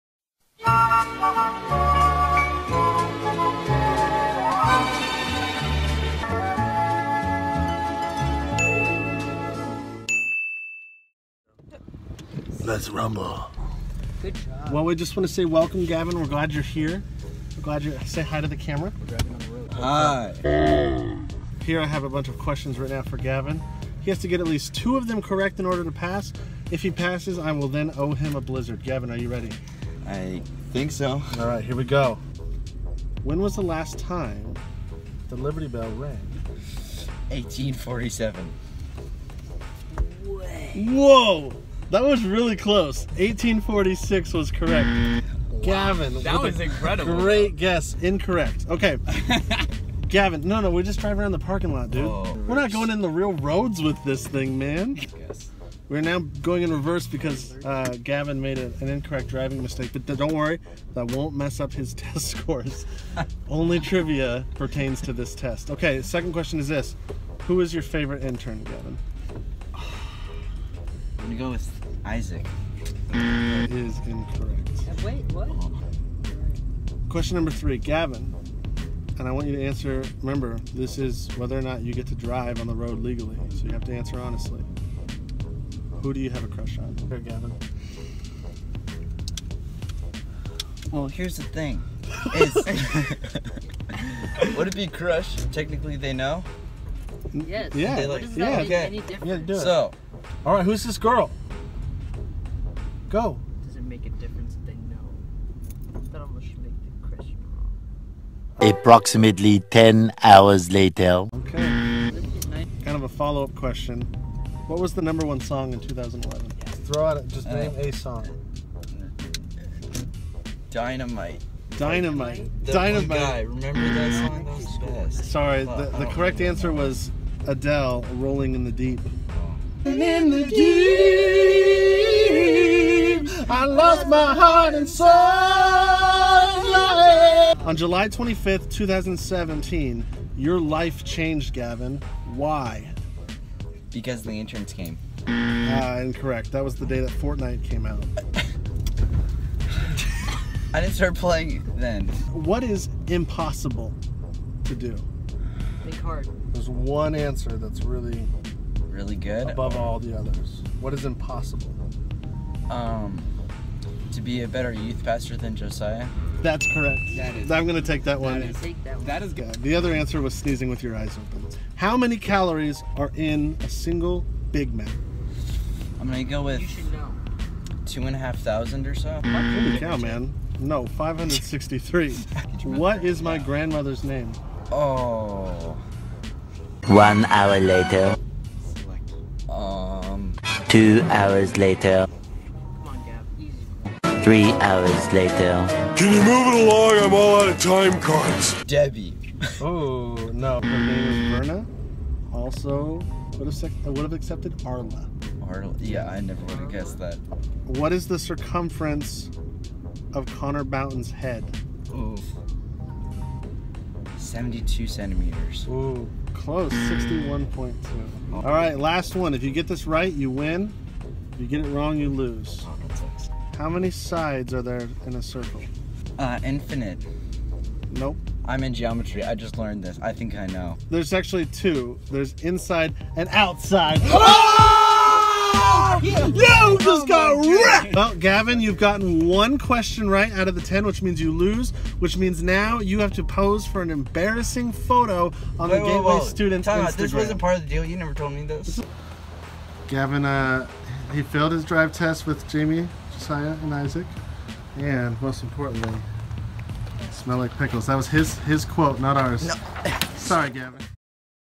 FIRST DRIVE- GAVIN SCHMIDT
Gavin takes a spin with Pastor Josiah and the Interns!